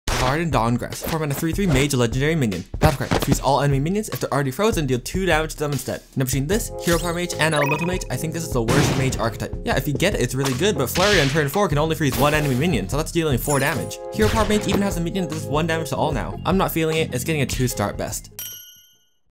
Quick Hearthstone Card Review: Varden Dawngrasp
Hearthstone just announced it's new expansion: Hearthstone Forged in the Barrens. This quick card review I go through the Hearthstone card Varden Dawngrasp to see how good the card will be in the upcoming Hearthstone meta and the year of the gryphon

If you are reading this I hope you dawngrasp whatever you're doing and dawngrasp it well.